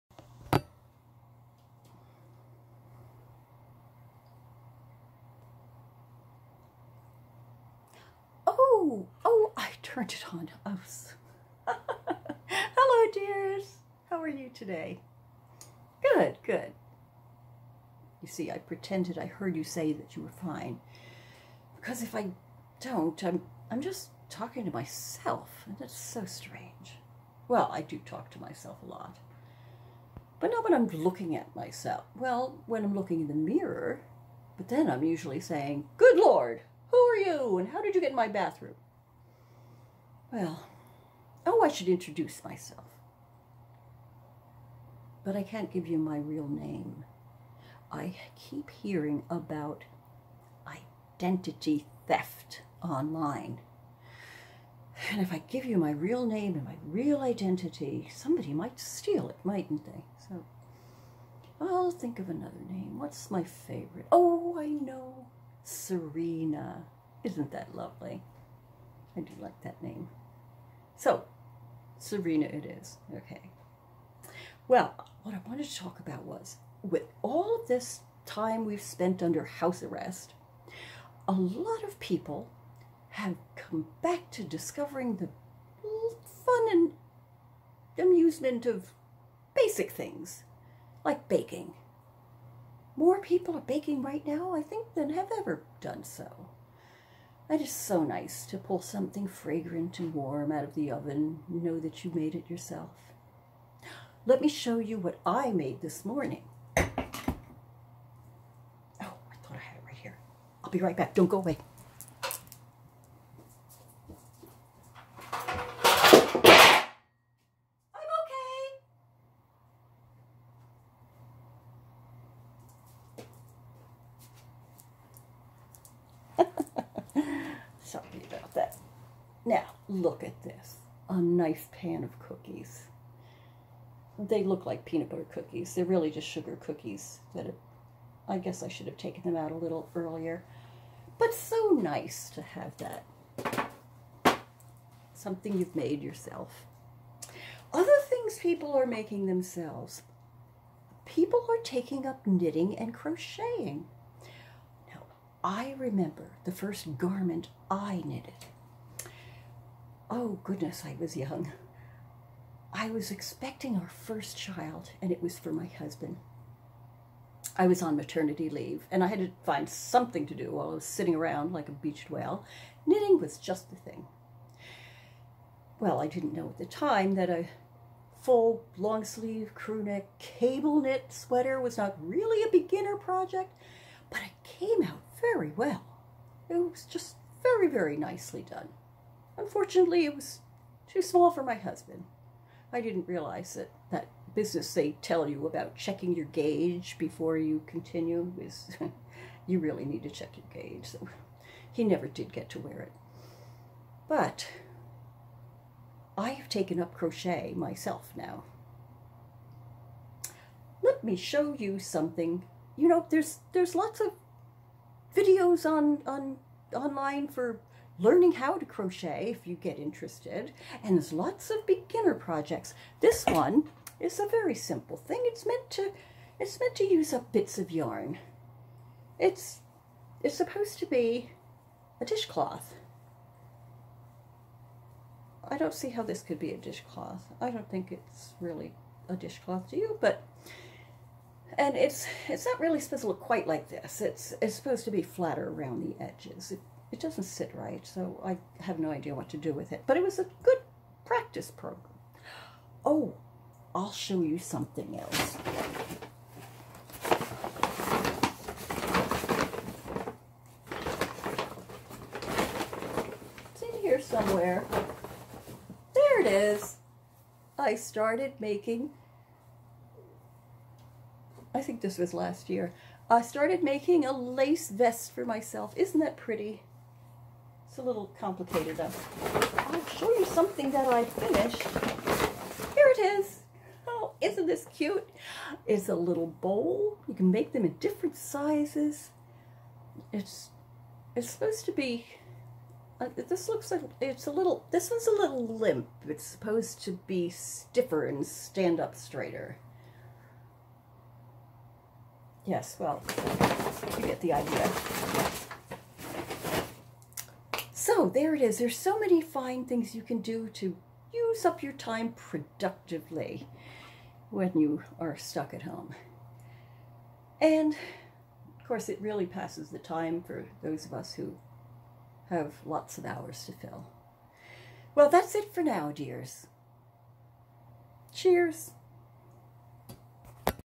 Introducing crafty "Serena"
The Old Bat introduces "Serena" and tries to inspire us with some interesting cookies and some yarn.